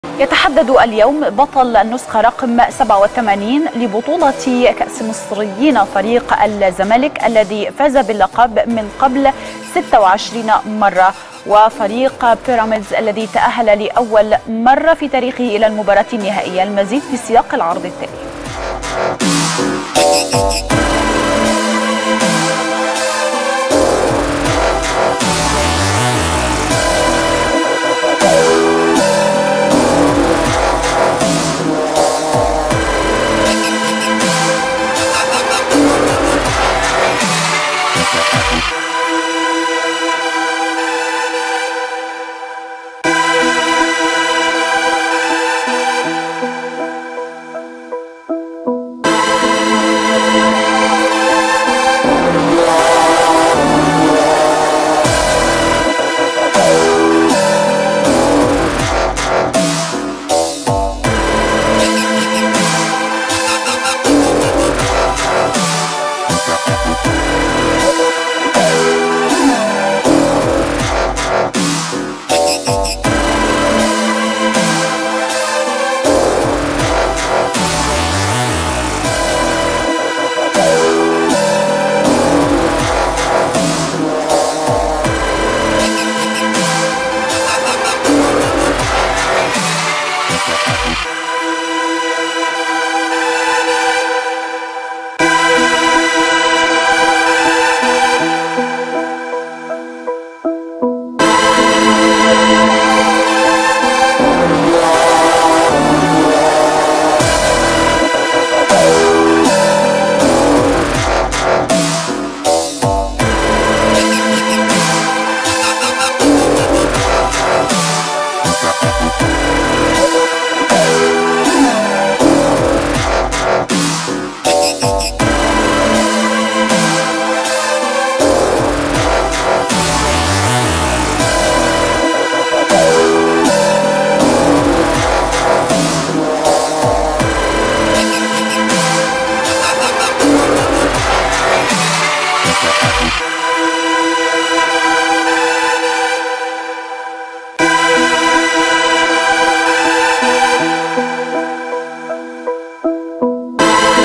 غرفة الأخبار | الزمالك يلاقي بيراميدز اليوم في النهائي.. تعرف أكثر على بطولة كأس مصر
غرفة الأخبار | من جولة الــ 12 ظهرًا الإخبارية مع همام مجاهد وإيمان الحويزي | 8 سبتمبر 2019